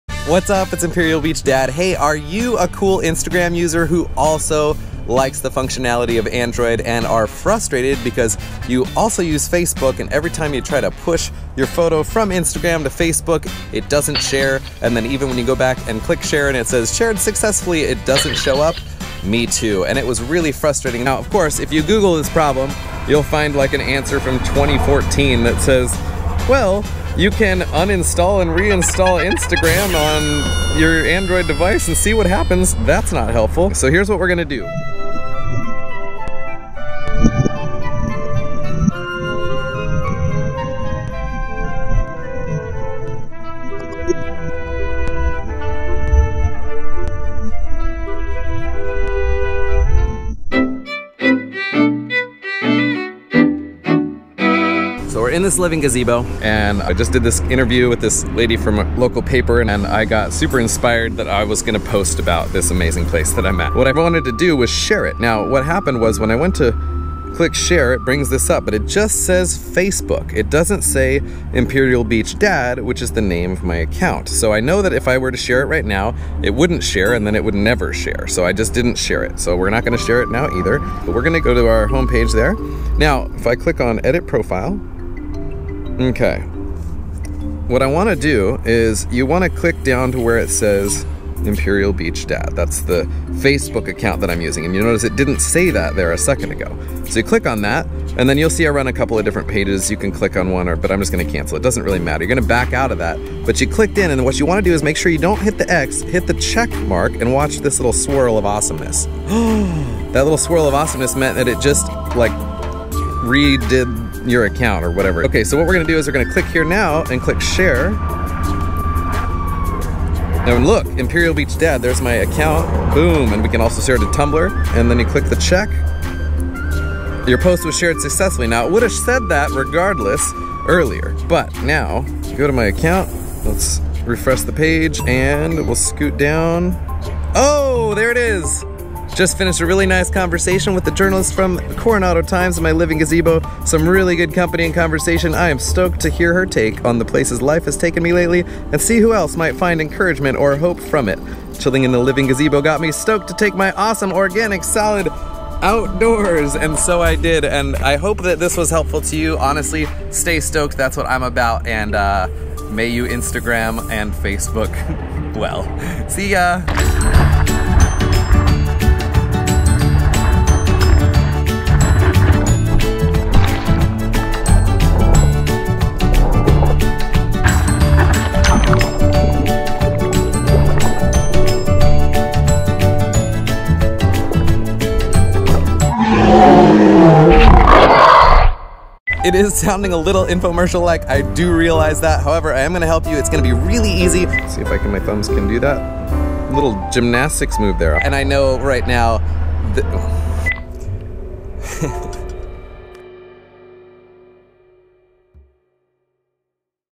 Instagram NOT sharing to Facebook??? PROBLEM SOLVED!!!
Frustrated because Instagram isn't sharing to Facebook the way it should?  I was too!!!  I reinstalled both apps and was still having trouble - sometimes it would share sometimes it wouldn't - until I discovered this super easy trick that works!

Follow the steps in this video to know your accounts are linked correctly before you click share and know that your instagram posts will reach your facebook friends as well.

This video is a bit out of my niche but I hope it helps some people out!  You can expect videos at least twice per week!